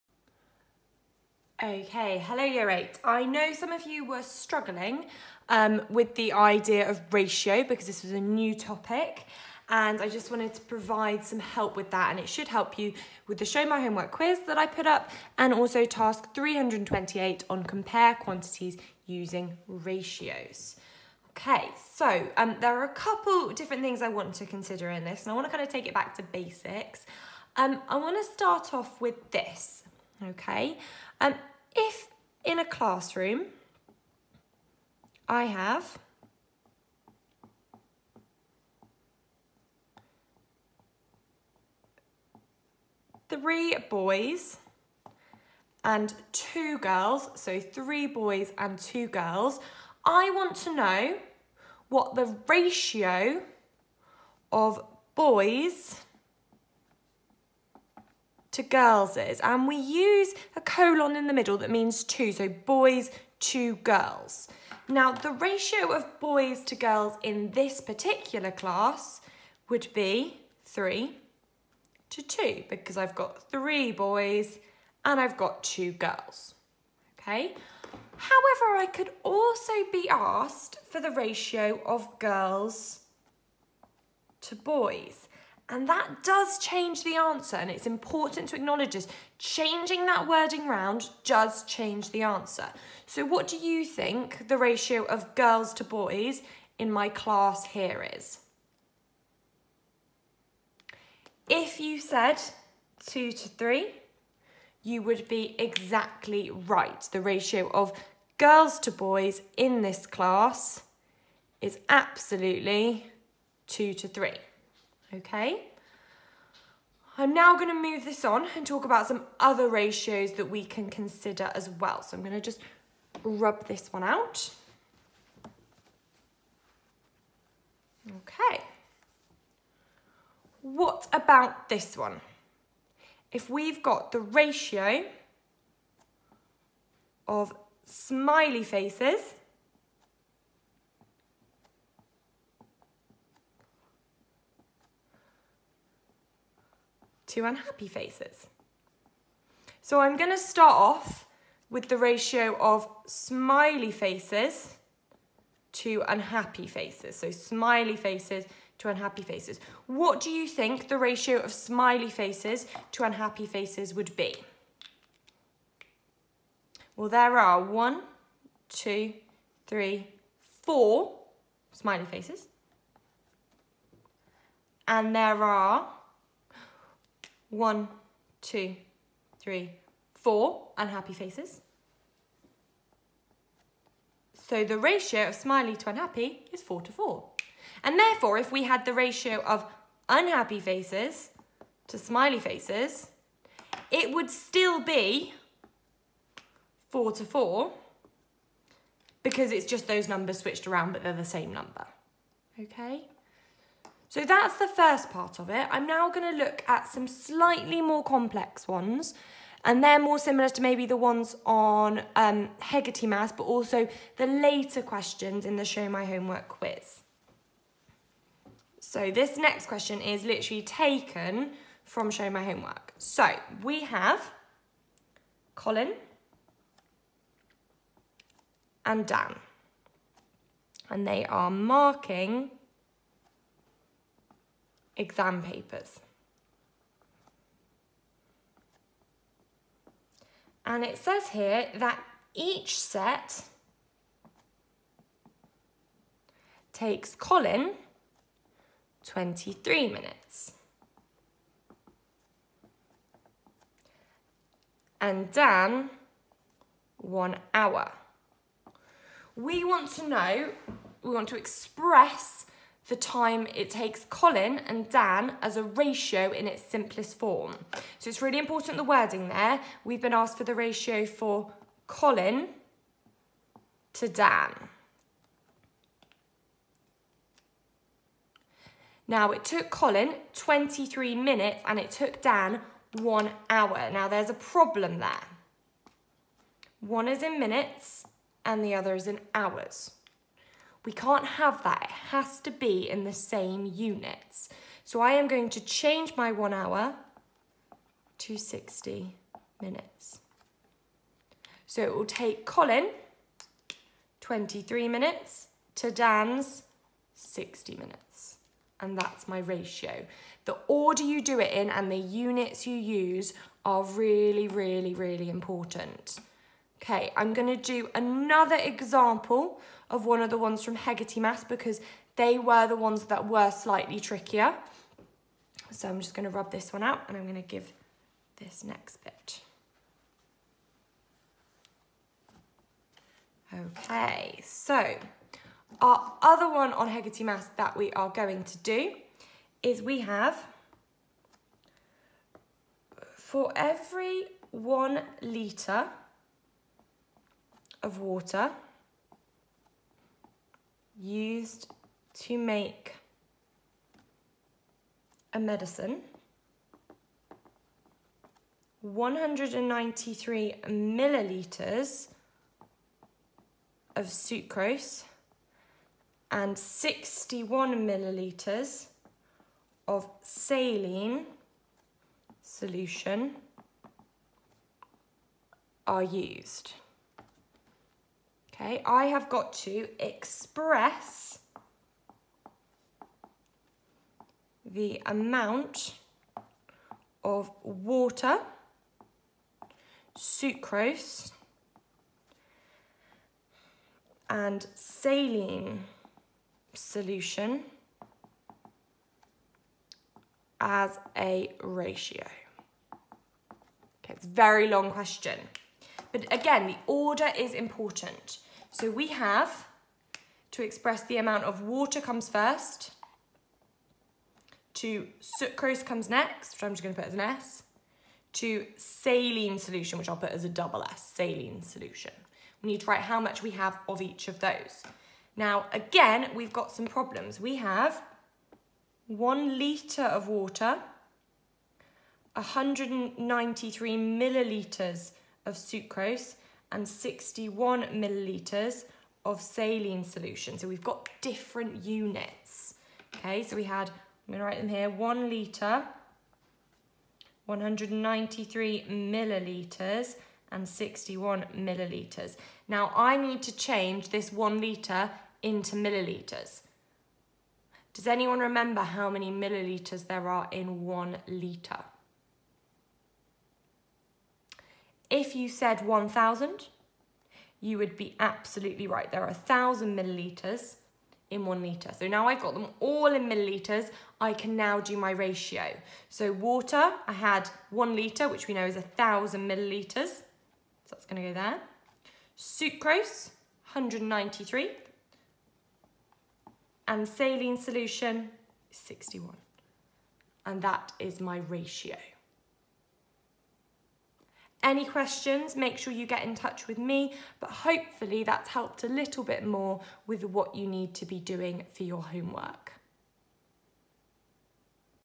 Compare quantities using ratios Examples
To help with the SMHW quiz and the Hegarty Maths task 328 - Compare quantities using ratios.